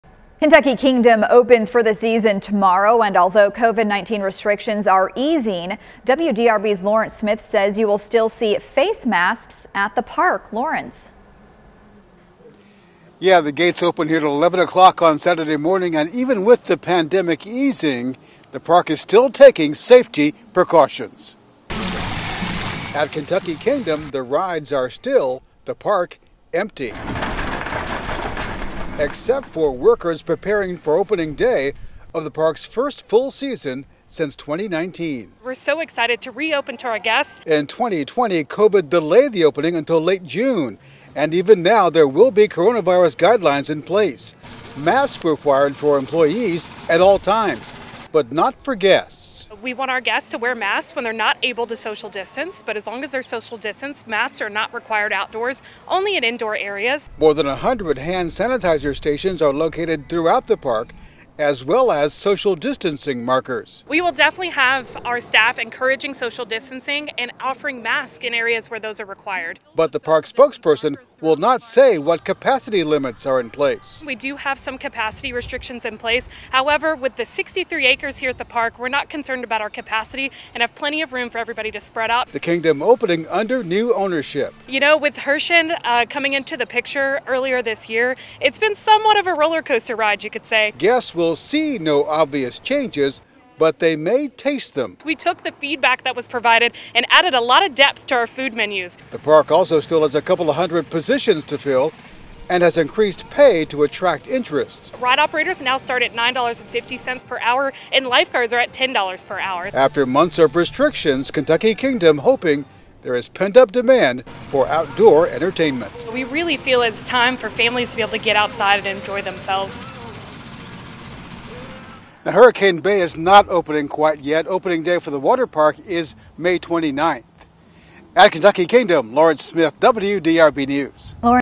Kentucky Kingdom to open for 34th season
Kentucky Kingdom is scheduled to open for its 34th season on Saturday, May 8.

It is the first season for new majority owners and operators, Herschend Family Entertainment, who joined the team in March.

The park has more than 70 rides, including six coasters, two wave pools and one of the tallest body slides in the world.